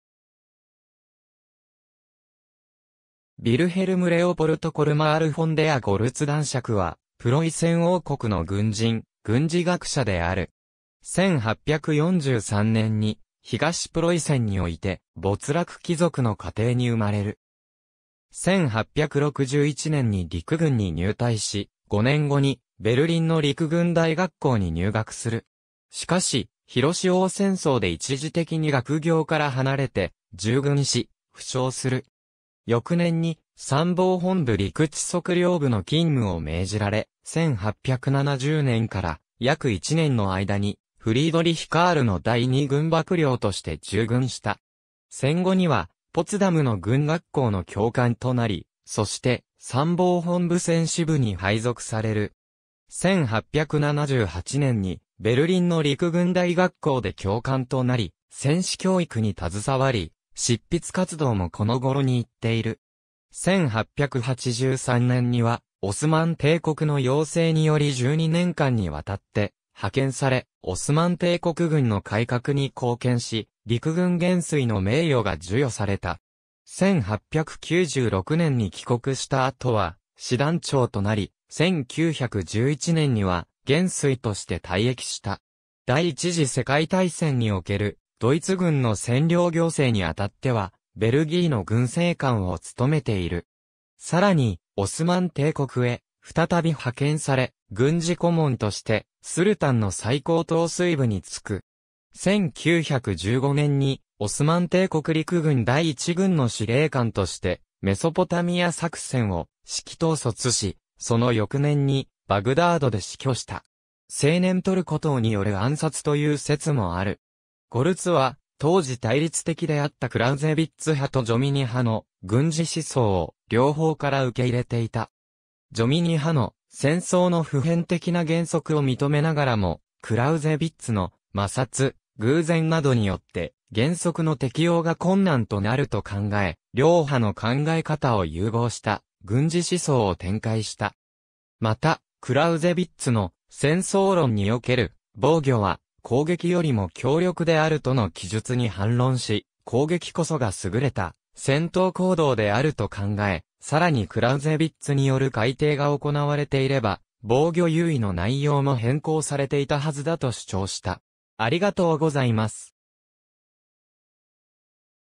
コルマール・フォン・デア・ゴルツ
ヴィルヘルム・レオポルト・コルマール・フォン・デア・ゴルツ男爵（Wilhelm Leopold Colmar Freiherr von der Goltz, 1843年8月12日 - 1916年4月19日）は、プロイセン王国の軍人、軍事学者である。
1843年に東プロイセンにおいて没落貴族の家庭に生まれる。
1861年に陸軍に入隊し、5年後にベルリンの陸軍大学校に入学する。
しかし普墺戦争で一時的に学業から離れて従軍し、負傷する。
翌年に参謀本部陸地測量部の勤務を命じられ、1870年から約1年の間にフリードリヒ・カールの第二軍幕僚として従軍した。
戦後にはポツダムの軍学校の教官となり、そして参謀本部戦史部に配属される。
1878年にベルリンの陸軍大学校で教官となり、戦史教育に携わり、執筆活動もこの頃に行っている。
1883年にはオスマン帝国の要請により12年間にわたって派遣され、オスマン帝国軍の改革に貢献し、陸軍元帥の名誉が授与された。
1896年に帰国した後は師団長となり、1911年には元帥として退役した。
第一次世界大戦におけるドイツ軍の占領行政に当たってはベルギーの軍政監を務めている。
さらにオスマン帝国へ再び派遣され、軍事顧問としてスルタンの最高統帥部に就く。
1915年にオスマン帝国陸軍第一軍の司令官としてメソポタミア作戦を指揮統率し、その翌年にバグダードで死去した。
青年トルコ党による暗殺という説もある。
ゴルツは当時対立的であったクラウゼヴィッツ派とジョミニ派の軍事思想を両方から受け入れていた。
ジョミニ派の「戦争の普遍的な原則」を認めながらも、クラウゼヴィッツの「摩擦」、「偶然」などによって原則の適用が困難となると考え、両派の考え方を融合した軍事思想を展開した。
またクラウゼヴィッツの『戦争論』における「防御は攻撃よりも強力である」との記述に反論し、攻撃こそがすぐれた戦闘行動であると考え、さらにクラウゼヴィッツによる改訂が行われていれば防御優位の内容も変更されていたはずだと主張した。